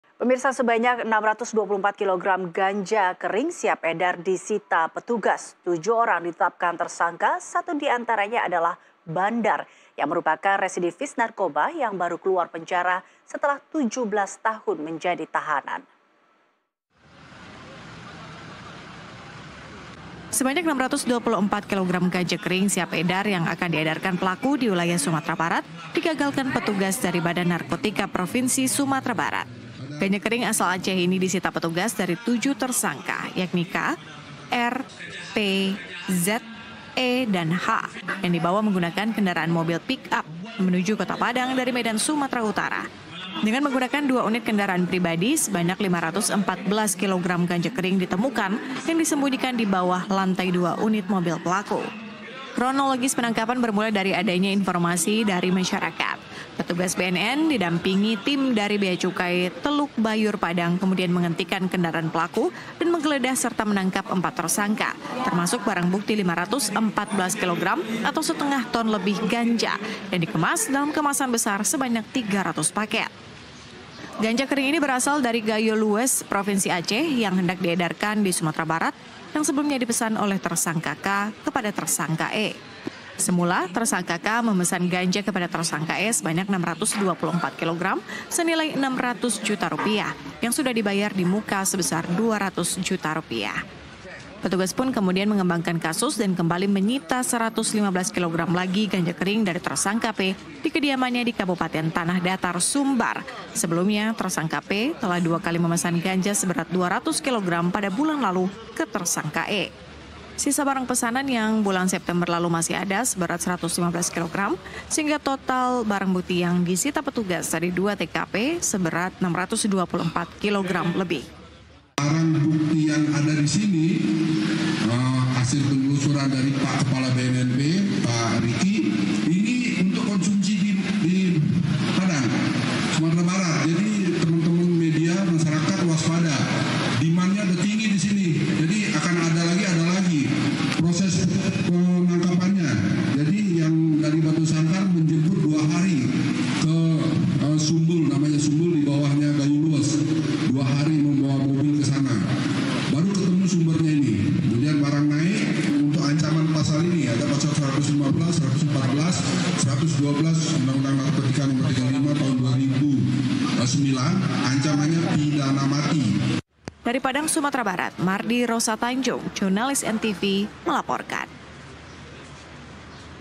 Residivis Narkoba Bebas, Langsung Kendalikan 624 Kg Ganja di Sumbar | NTV CRIME
BNNP Sumatera Barat berhasil menyita 624 kg ganja kering siap edar yang akan didistribusikan di Sumatera Barat. Tujuh orang ditetapkan sebagai tersangka, termasuk seorang bandar residivis yang baru bebas setelah 17 tahun dipenjara.

Ganja asal Aceh ini ditemukan dalam dua kendaraan yang menuju Padang dari Medan. Sebanyak 514 kg disembunyikan di mobil, sementara 115 kg lainnya ditemukan di rumah tersangka di Tanah Datar. Para tersangka dijerat dengan Undang-Undang Narkotika Nomor 35 Tahun 2009, dengan ancaman hukuman mati.

ENNTV